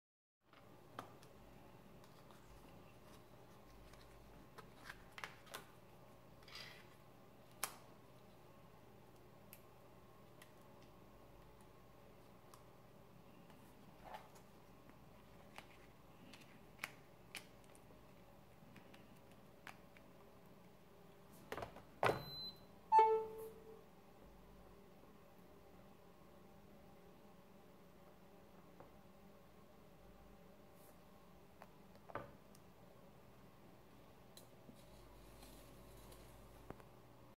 SimpleMatch Card
Mit der SimpleMatch® Card, kannst Du ebenfalls kabellos laden! Diese steckst Du einfach hinter das Cover deines Handy's. ...und schon geht's los!